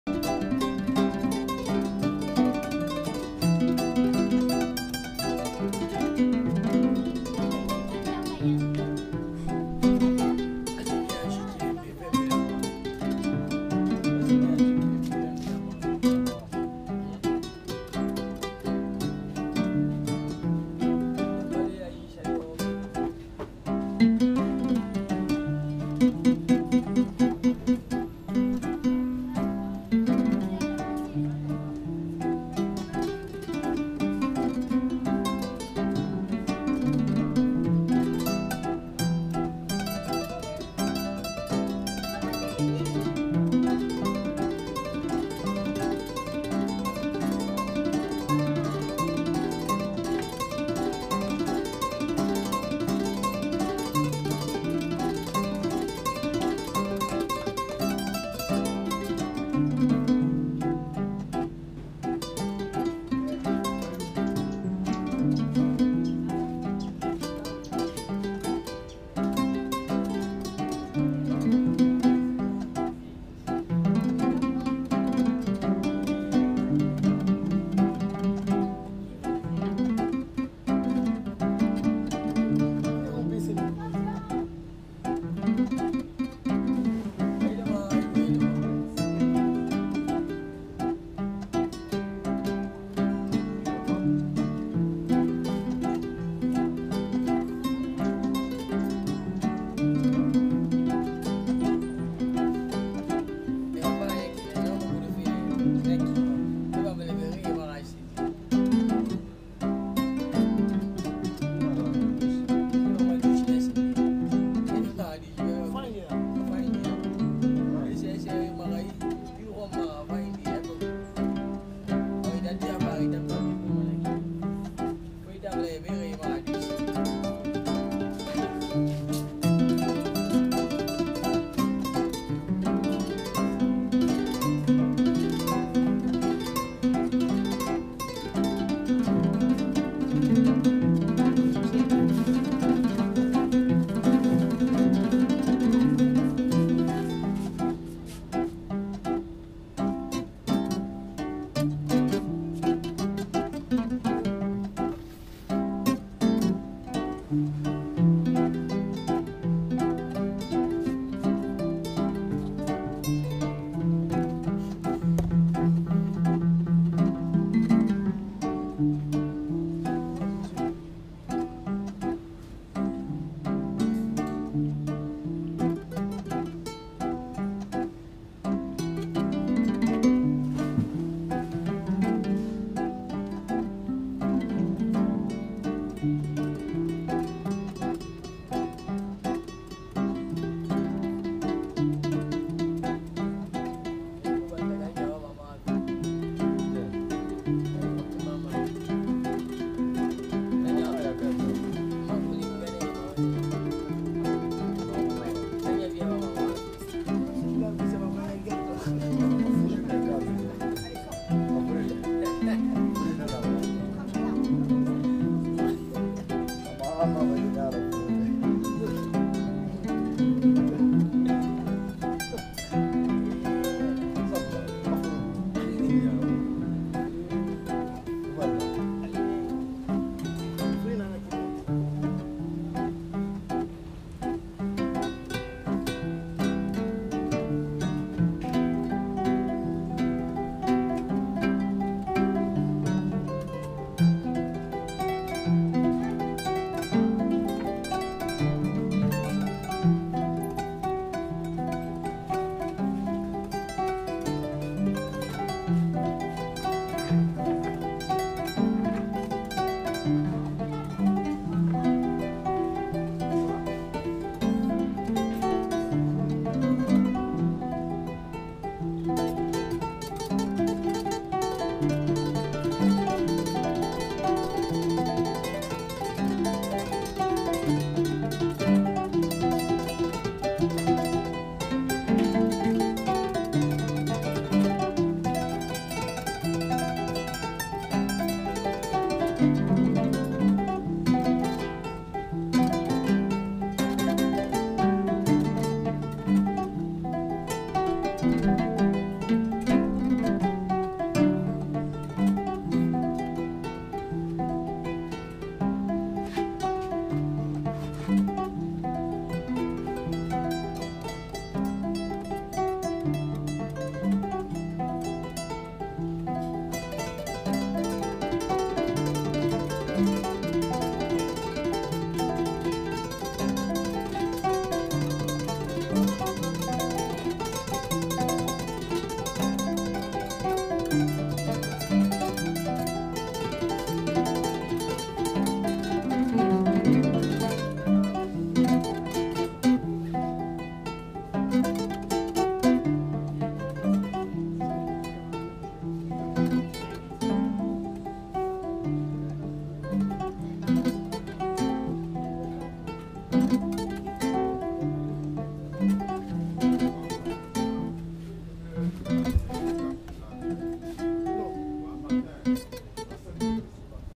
kora Séfoudy Kouyaté et M'Bady Diabaté
séfoudy Kouyaté et M'bady Diabaté ,la nouvelle génération des korafolas...

Séfoudy est le korafola des espoirs de Corontie ,c'est le fils de M'bady Kouyaté ,reconnu par tous comme le dernier grand maitre de kora, directeur artistique du ballet nationnal Guinéen pendant des années.
notez son jeu particulier a trois doigts et sa rapidité a seulement 17 ans, ca promet...